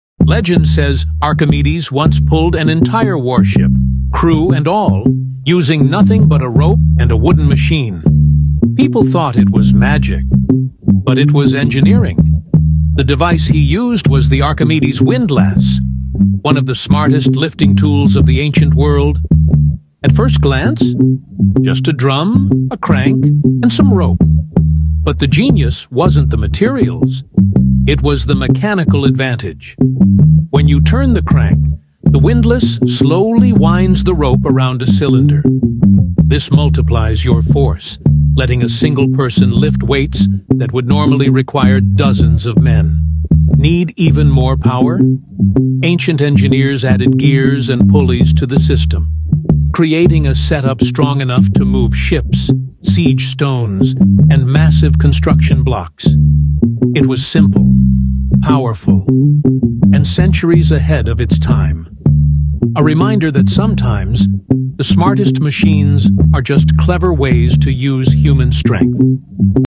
adiaava Archimedes Windlass Device Used To Lift Heavy Objects
Discover the incredible ancient machine that made a single man strong enough to move a warship — the Archimedes Windlass. This 2,000-year-old engineering invention used mechanical advantage to lift massive weights with almost no effort.
In this short video, we break down how the windlass worked, why Archimedes shocked the ancient world with it, and how the same principles still power cranes, winches, and modern lifting machines today.